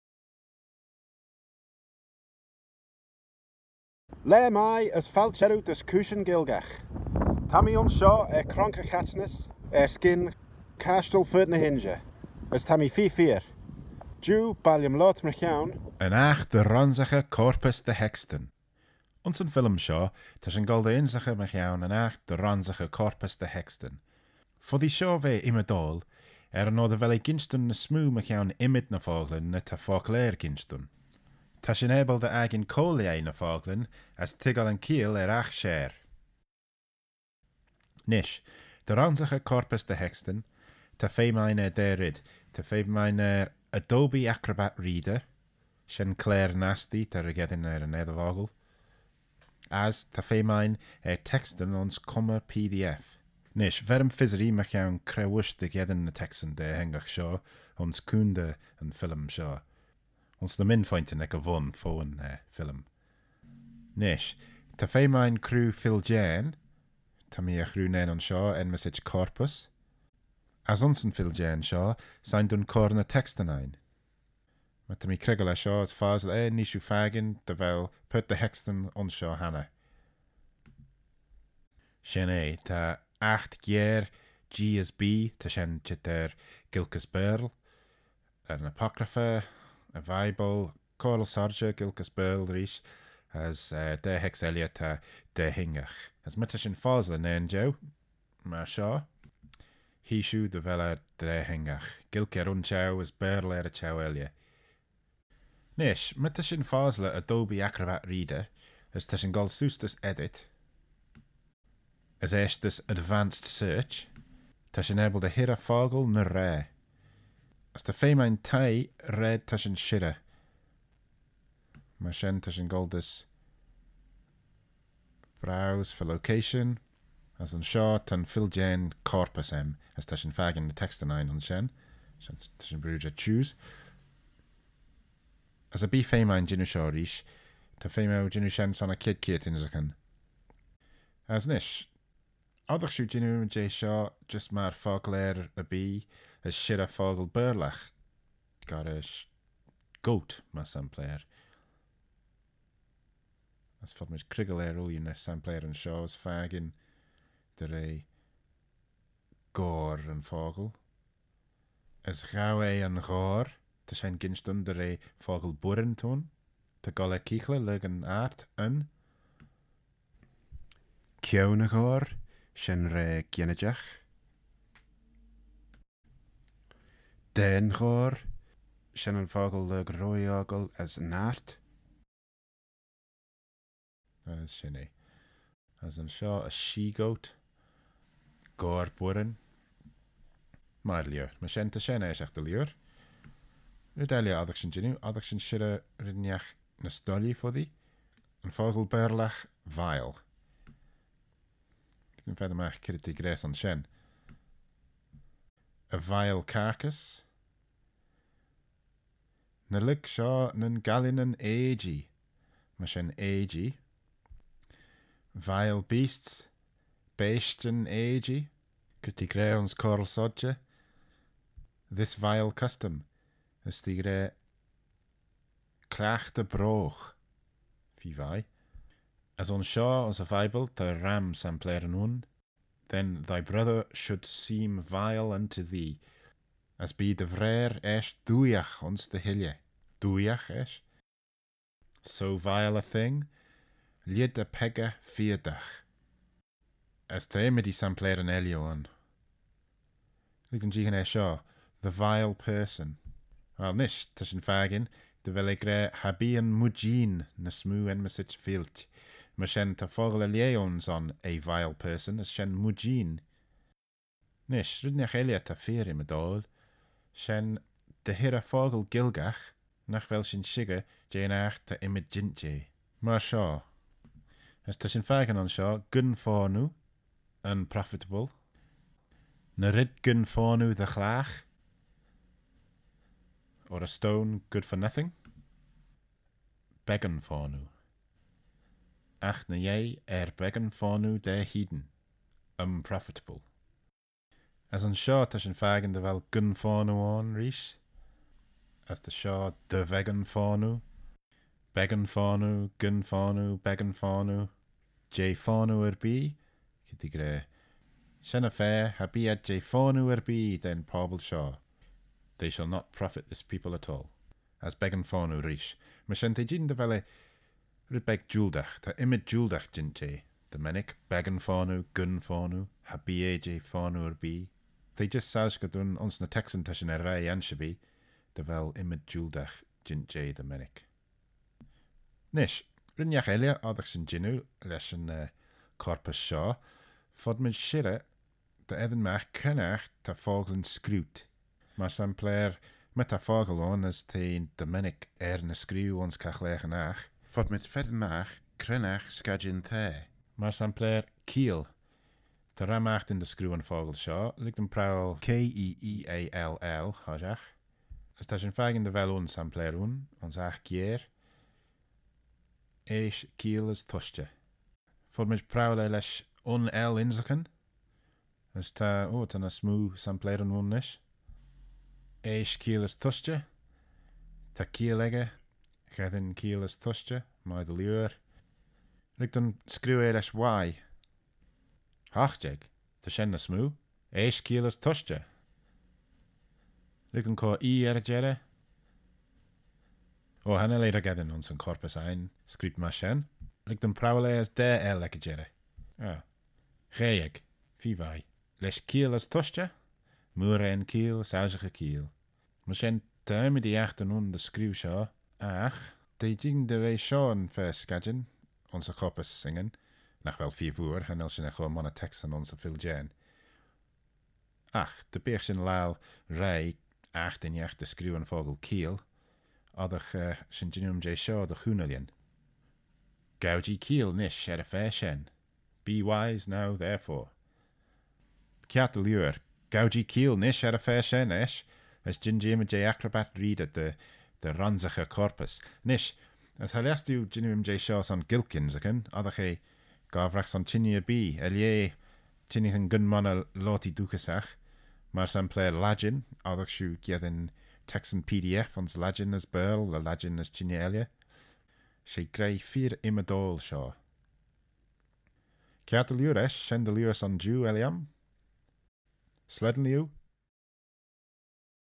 Cooishyn Gailckagh 1 - How to search the Corpus of Manx Gaelic Texts
Fillym ayns Gailck mychione yn aght dy ronsaghey corpus dy hextyn.

A film in Manx Gaelic about how to search through a corpus of texts.

Do you want to learn Manx?